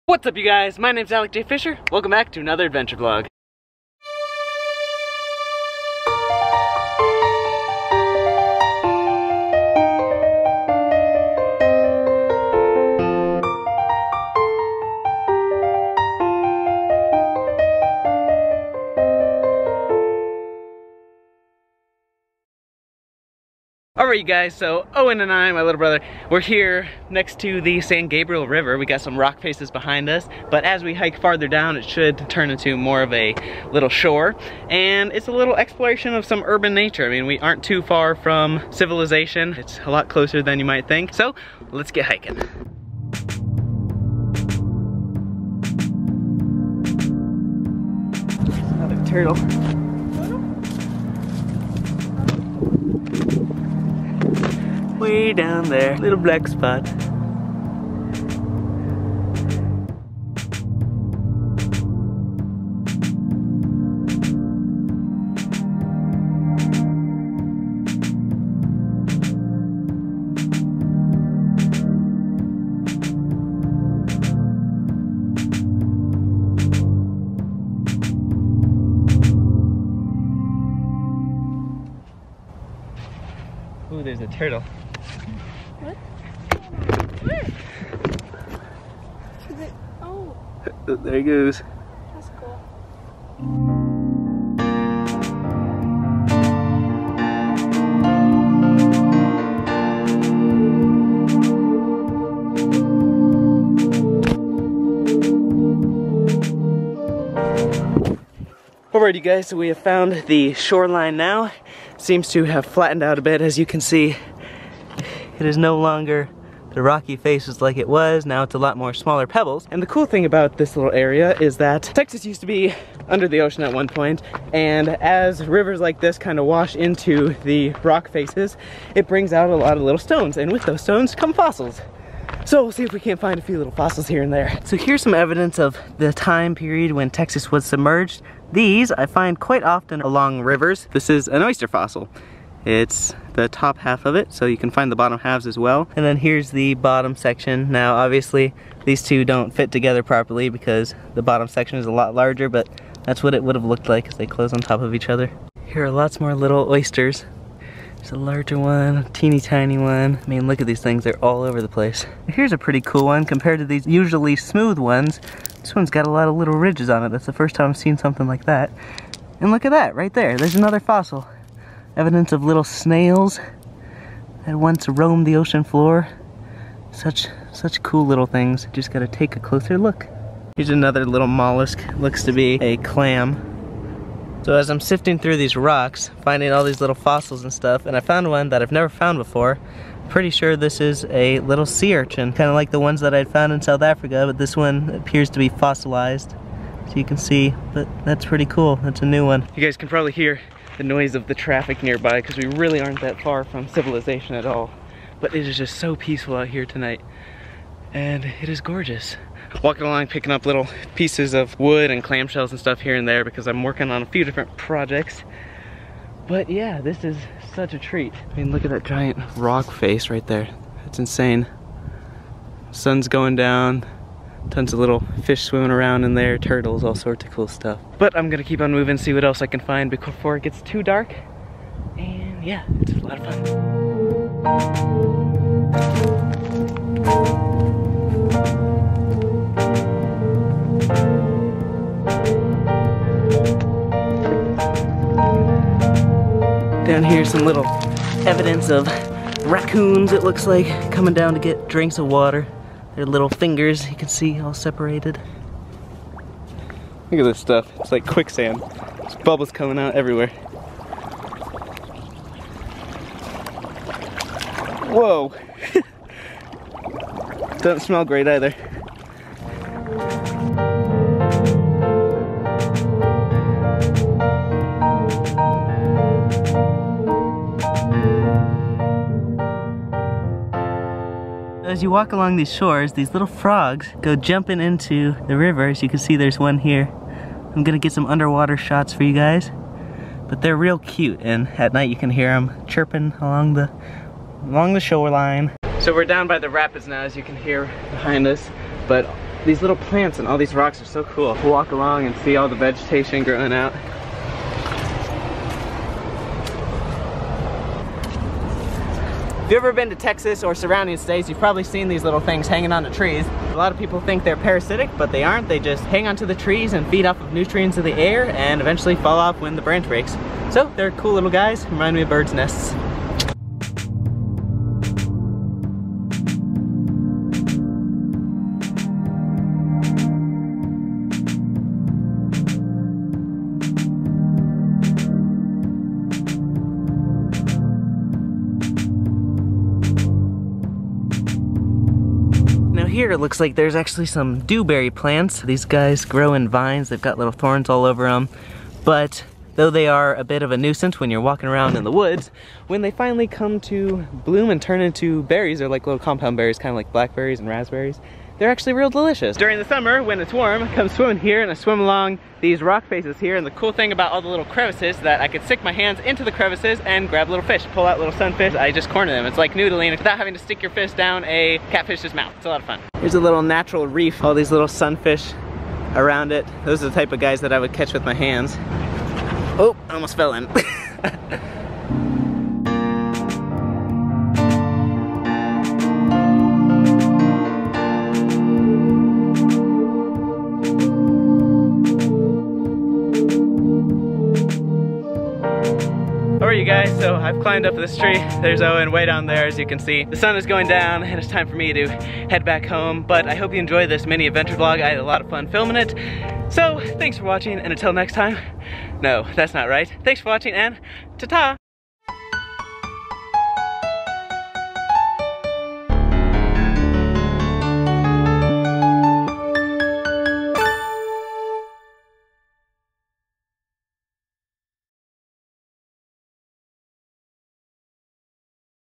Fossils, Frogs and Photography | Adventure Vlog #35
I took an evening hike along the San Gabriel river to see what I could find! I uncovered a number of assorted fossils, found some frogs and invertebrates and more than anything, lots of beautiful views! I had a blast making it! Enjoy! :)